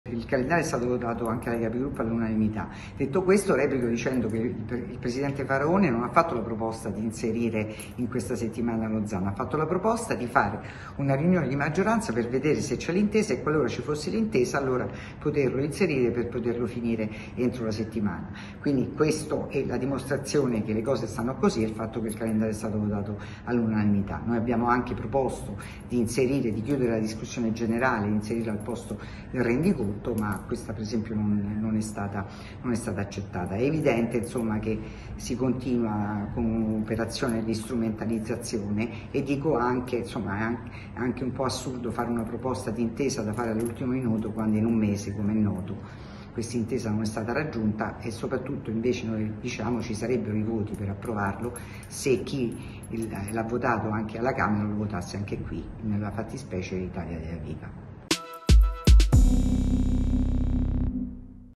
Ddl Zan, De Petris: Iv strumentalizza, con suoi voti sarebbe approvato
"E' evidente che si continua con un'operazione di strumentalizzazione" . Così la capogruppo Leu, Loredana De Petris commentando la proposta avanzata dal senatore di Italia viva Davide Faraone, che aggiunge: "Ci sarebbero i voti per votare il provvedimento se chi lo ha votato alla Camera lo votasse anche al Senato, nella fattispecie Italia Viva". (sca)